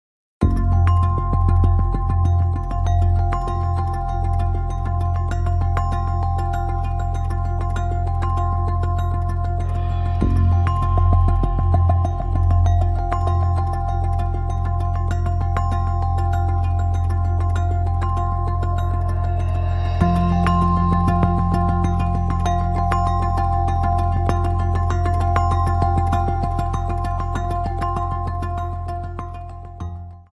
Benzin istasyonunda yürüyen yayaya araçla çarptı
İstanbul Zeytinburnu'nda benzin istasyonuna hızla giren otomobil bir kişiye çarparak kaçtı. O anlar kameralara yansıdı.